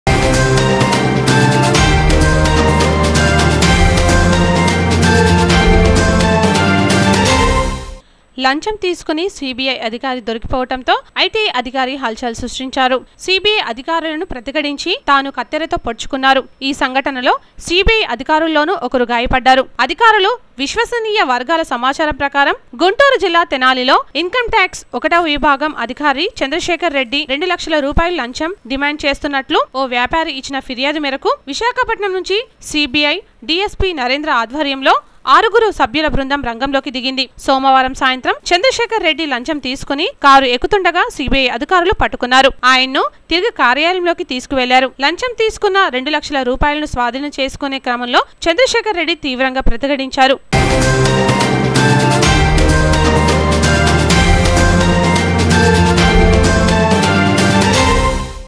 GUNTUR JILLA TENALILO LAUNCHAM TISUKUNTU PATTUBADDA IT ADHIKARI HULCHUL
లంచం తీసుకుని సీబీఐకి దొరికిపోయిన ఐటీ అధికారి హల్‌చల్‌ సృష్టించారు. సీబీఐ అధికారులను ప్రతిఘటించి... తాను కత్తెరతో పొడుచుకున్నారు. ఈ సంఘటనలో సీబీఐ అధికారుల్లోనూ ఒకరు గాయపడ్డారు. అధికారులు, విశ్వసనీయ వర్గాల సమాచారం ప్రకారం... గుంటూరు జిల్లా తెనాలిలో ఇన్‌కం ట్యాక్స్‌-1వ విభాగం అధికారి చంద్రశేఖర్‌ రెడ్డి రూ.2 లక్షలు లంచం డిమాండ్‌ చేస్తున్నట్లు ఒక వ్యాపారి ఇచ్చిన ఫిర్యాదు మేరకు విశాఖపట్నం నుంచి సీబీఐ డీఎస్పీ నరేంద్ర ఆధ్వర్యంలో ఆరుగురు సభ్యుల బృందం రంగంలోకి దిగింది. సోమవారం సాయంత్రం చంద్రశేఖర్‌ రెడ్డి లంచం తీసుకుని కారు ఎక్కుతుండగా సీబీఐ అధికారులు పట్టుకున్నారు. ఆయనను తిరిగి కార్యాలయంలోకి తీసుకువెళ్లారు. లంచంగా తీసుకున్న రూ.2 లక్షలను స్వాధీనం చేసుకునే క్రమంలో చంద్రశేఖర్‌ రెడ్డి తీవ్రంగా ప్రతిఘటించారు.